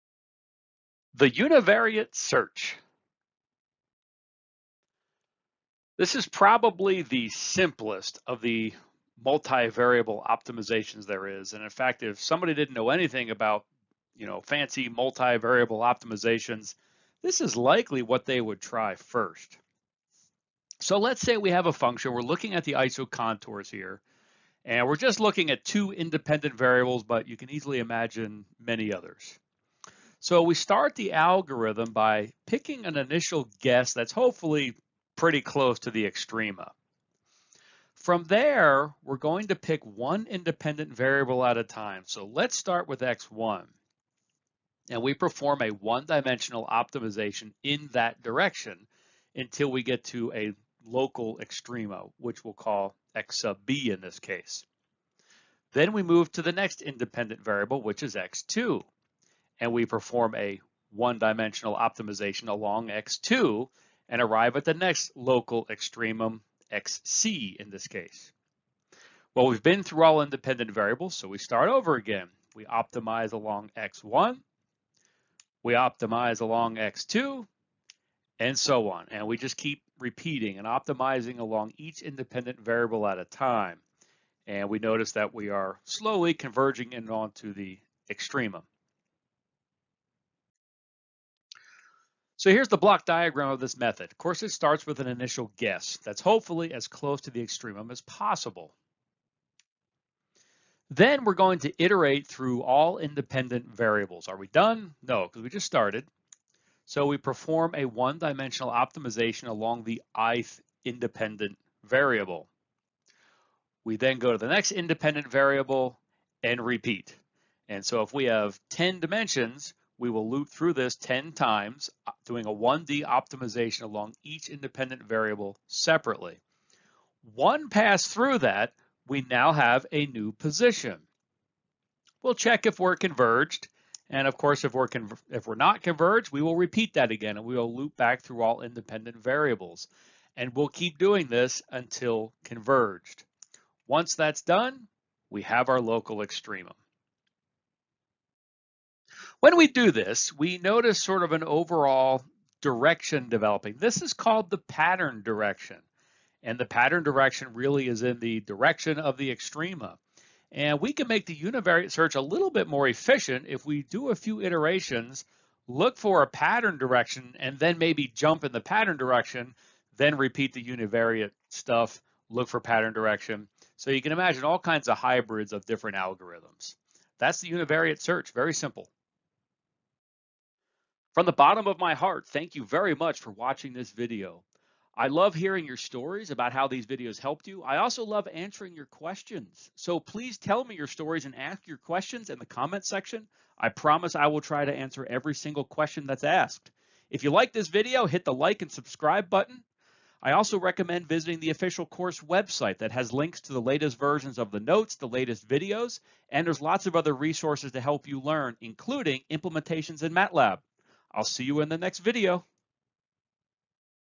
Lecture -- Univariate Search
This video explains the simplest multivariable optimization called the univariate search.  The method is illustrated and block diagram provided.

Be sure to visit the EMPossible Course website for updated lecture notes and course materials, as well as links to develop MATLAB codes.